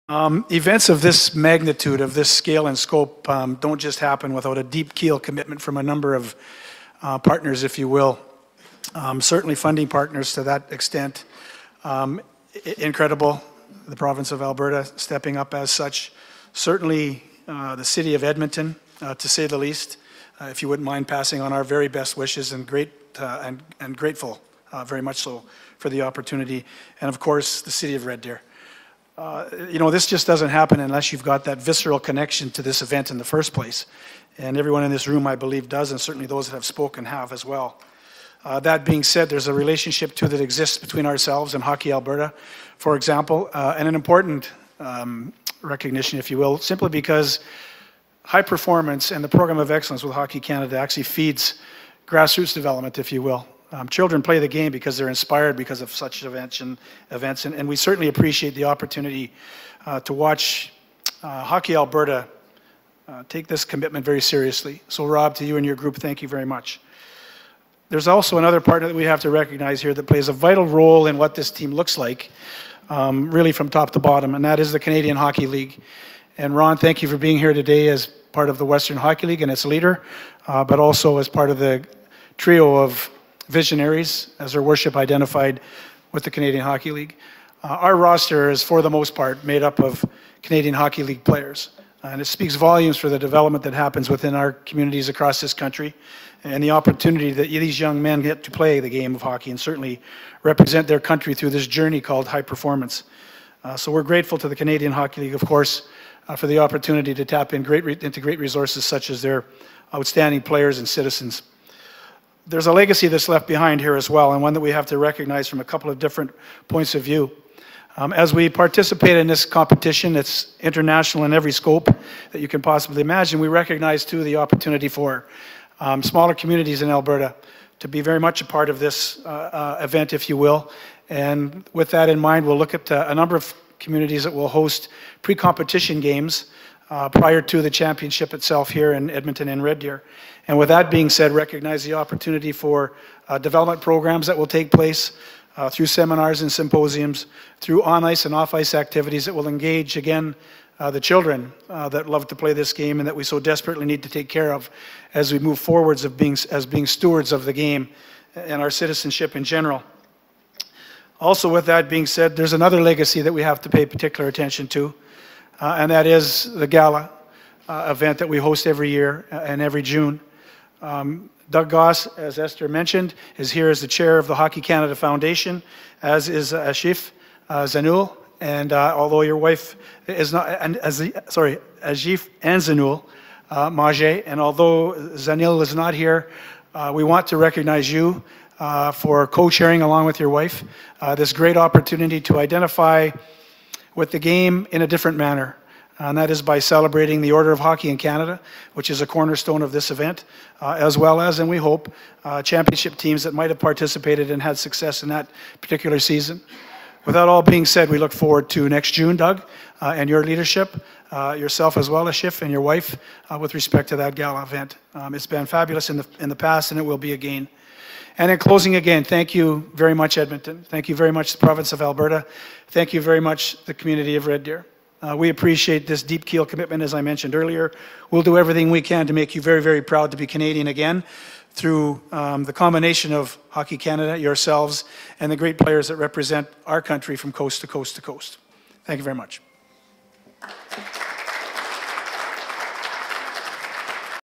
WJC 2021 | Tom Renney
Watch the press conference as Edmonton and Red Deer are announced as the locations for the 2021 World Junior Hockey Championships.